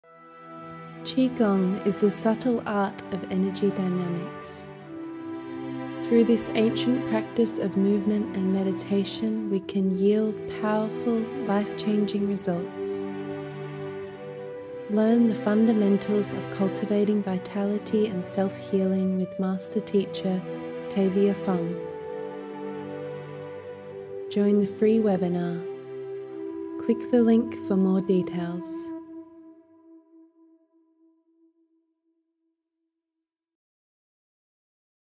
FREE Foundations of Qigong webinar from White Tiger Qigong
Join this FREE Foundations of Qigong webinar from White Tiger Qigong:
Within this webinar, White Tiger answers the questions around 'What is Qigong?', the ancient foundations of Qigong, and why it remains relevant for everyday modern life.

If you prefer to read instead:
Get a FREE 5 Element Qigong E-Book here: 👇

Please be sure to "Like" this video 👍